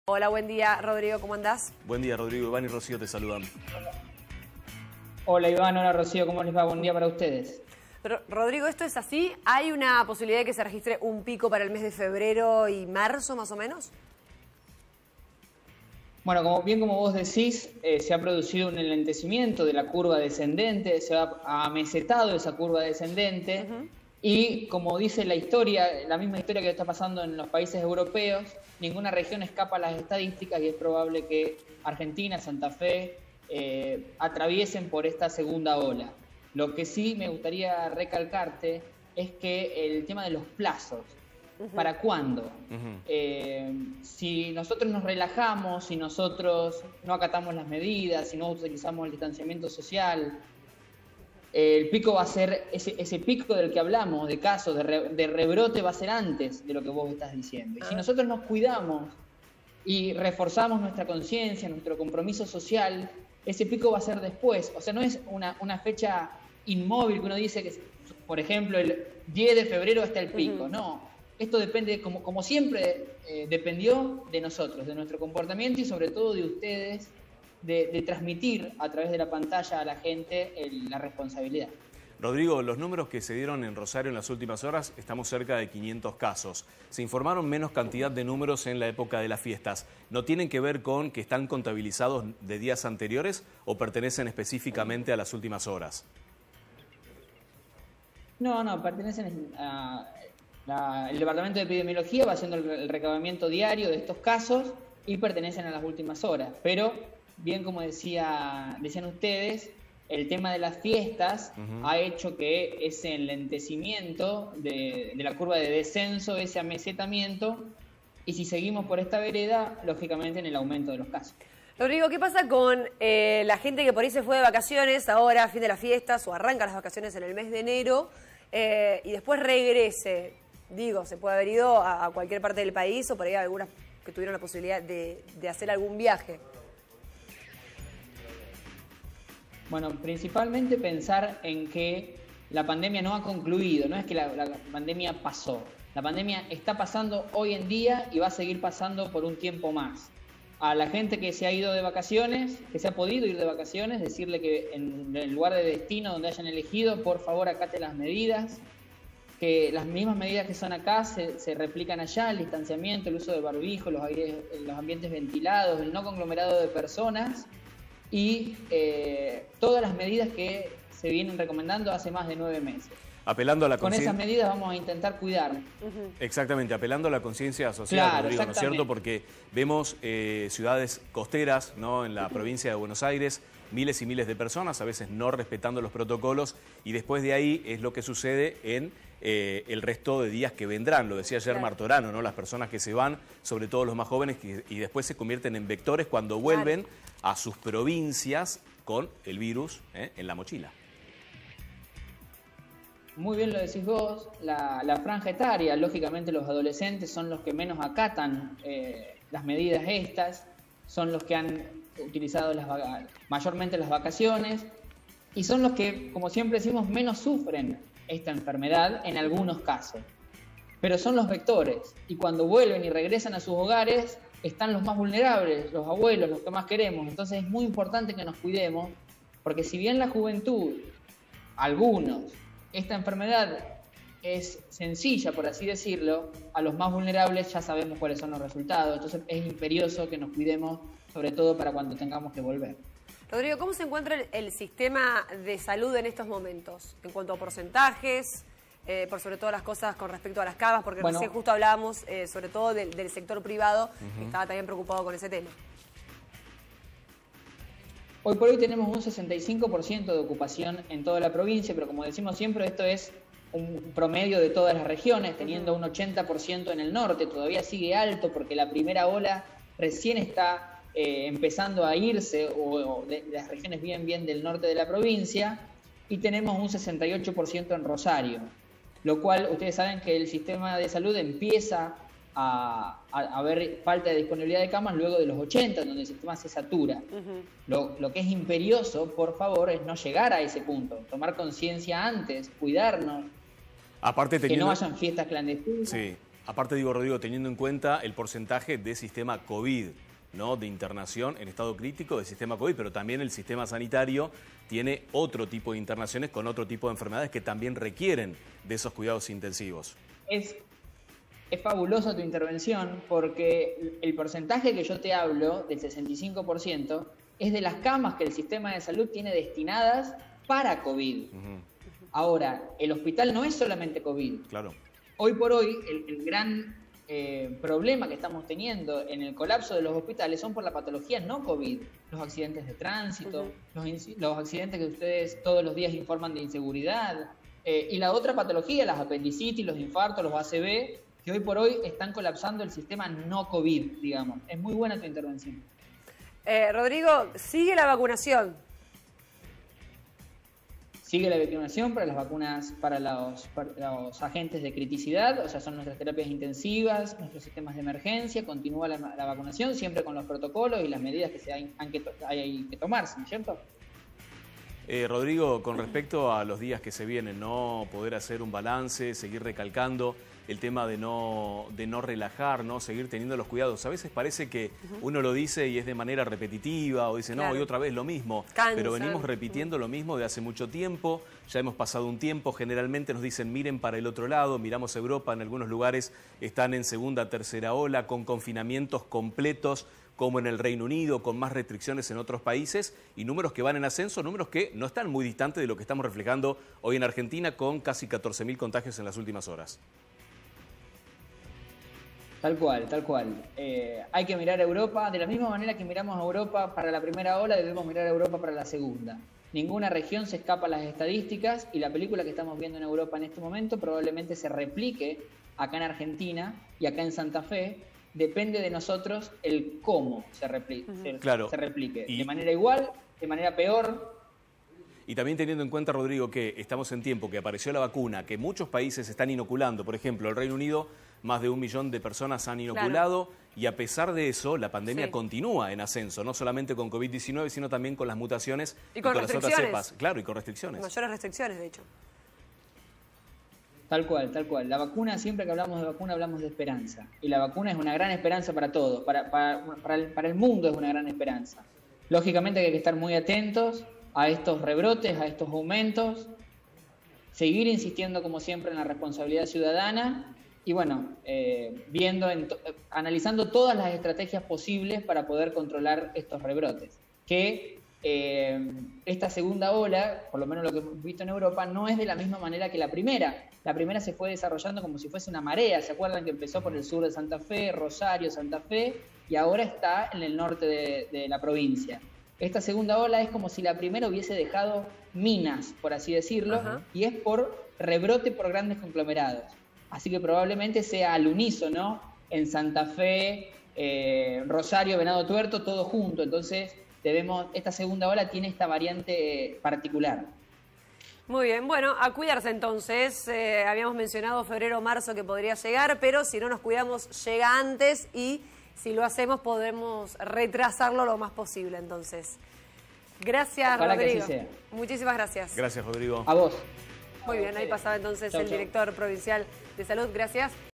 Segunda ola y nueva cepa de Covid en Argentina
El Director Provincial de Salud pidió extremar los controles y la responsabilidad individual para evitar un rebrote descontrolado de la enfermedad. Dijo que la vacuna es una esperanza pero no hay que dejar de cuidarse